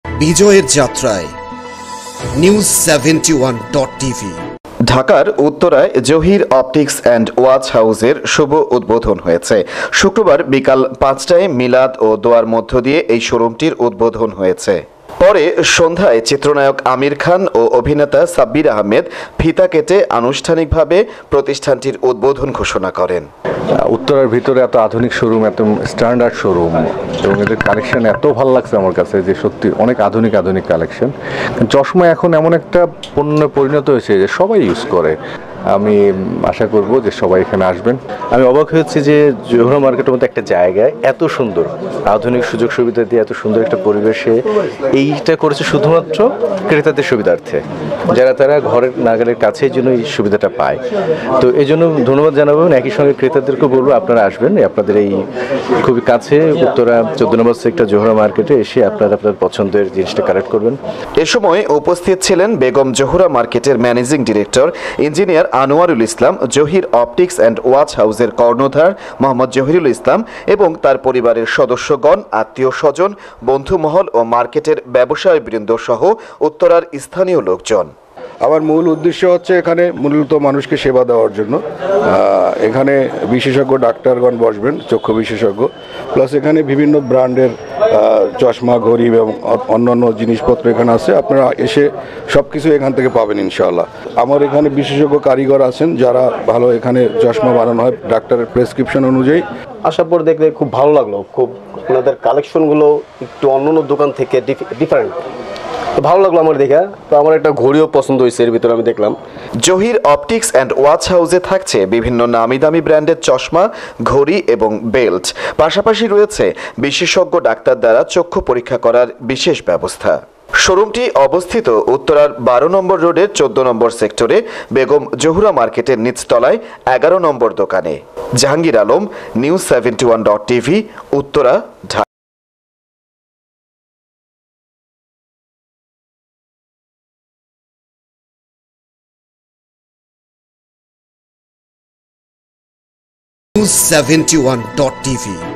ঢাকার উত্তরায় জহির অপটিকস এন্ড ওয়াচ হাউজ এর শুভ উদ্বোধন হয়েছে।
ঢাকার উত্তরায় জহির অপটিকস এন্ড ওয়াচ হাউজ এর শুভ উদ্বোধন হয়েছে। শোরুমটিতে থাকেছে বিভিন্ন নামিদামি ব্র্যান্ডের চশমা, ঘড়ি এবং বেল্ট। পাশাপাশি রয়েছে বিশেষজ্ঞ ডাক্তার দ্বারা চক্ষু পরীক্ষার বিশেষ ব্যবস্থা